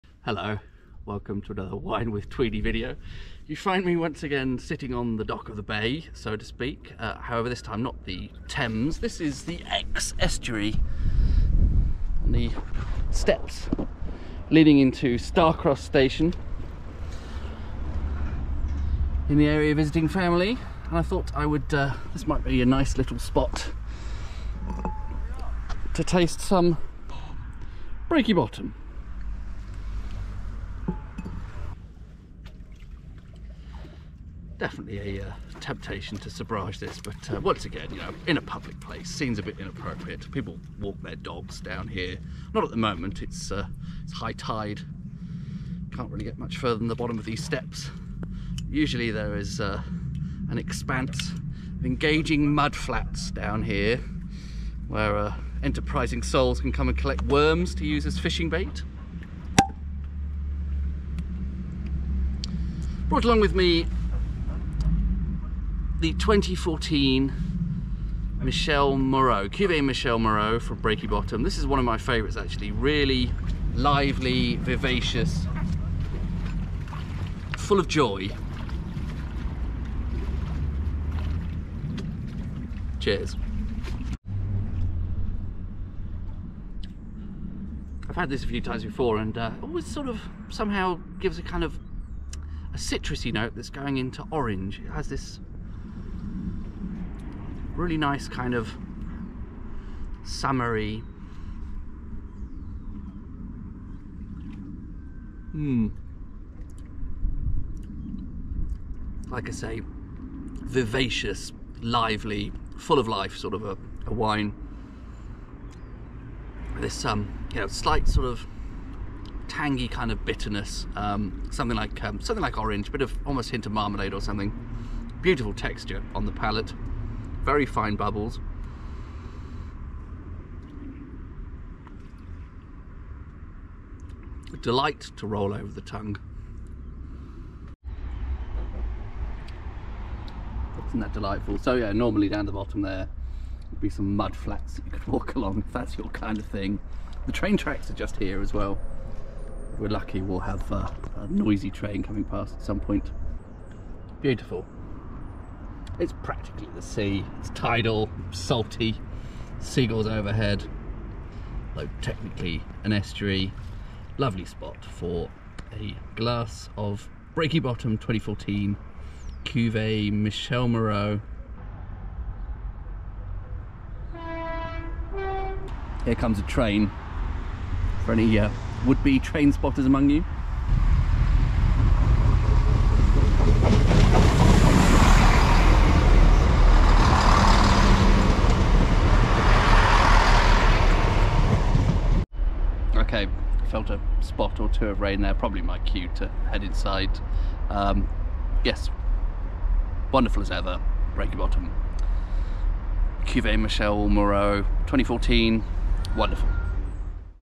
Breaky Bottom 2014 Cuvée Michelle Moreau on the Steps Down to the Exe
Filmed July 2023. On the steps leading down to the sea (well, technicall the Exe estuary) from Starcross station, near Exeter in Devon.

Tasting Breaky Bottom's 2014 vintage English sparkling wine made with traditional Champagne grapes. Bought directly from Breaky Bottom via their website.